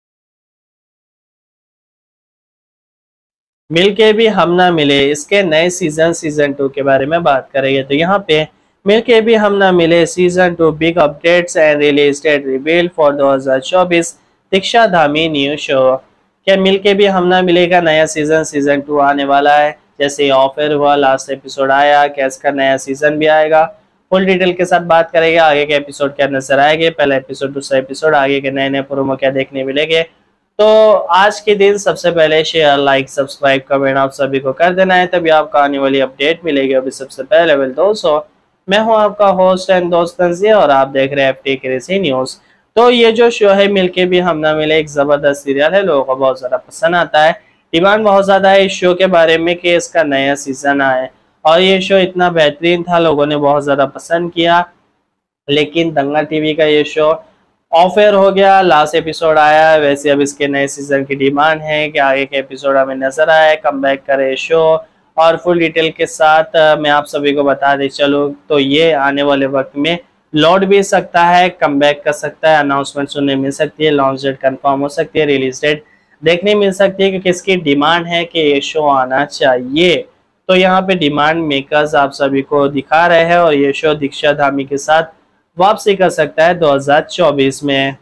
Mil ke bhi hum na mile Season 2 Big Updates & Release Date Revealed for 2024 | Diksha Dhami New Show
About this video Dosto Is Video Me Maine Aapko Mil ke bhi hum na mile Season 2 Big Updates & Release Date Revealed for 2024 | Diksha Dhami New Show Ummmed Hai Ye Video Aapko Bahut Pasand Aayegi.